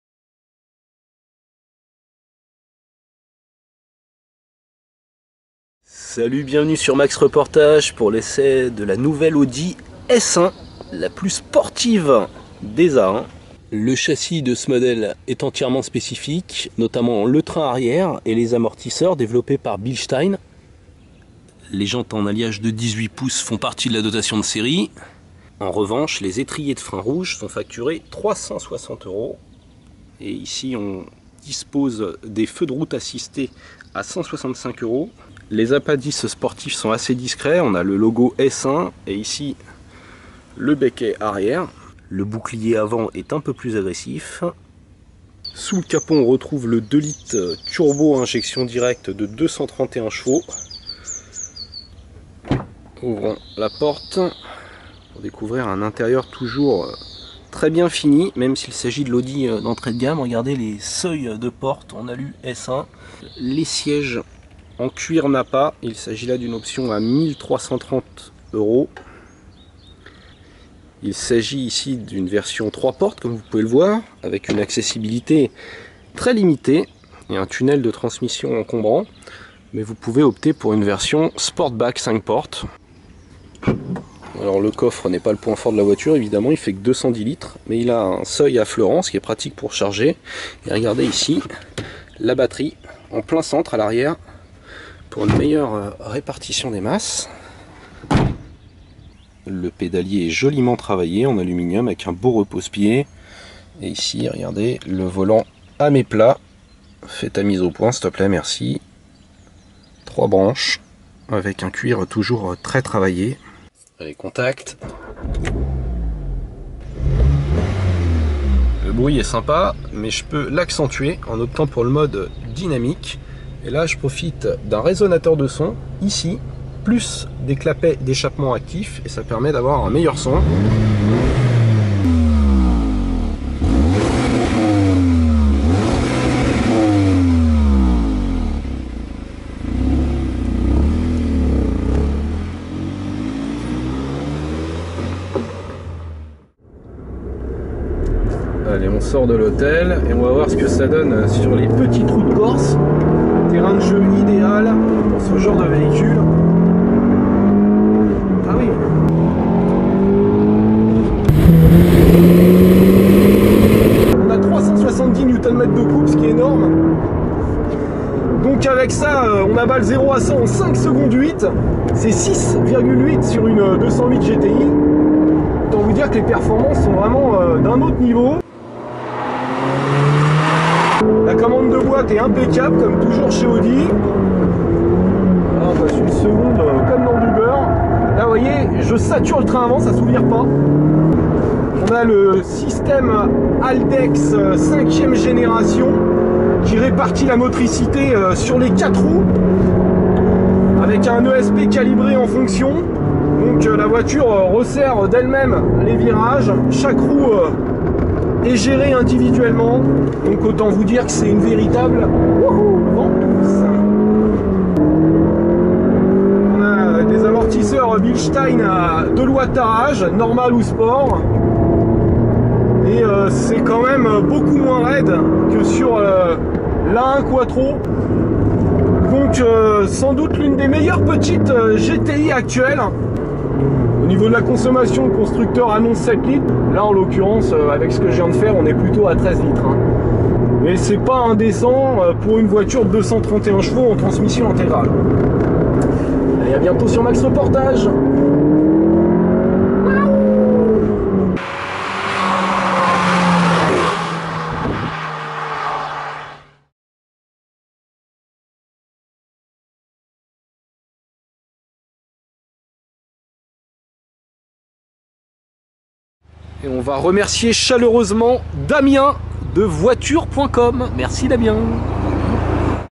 2014 Audi S1 Essai MAXreportage.com
Essai de l'Audi S1 2.0 TFSI quattro de 231 ch en Corse.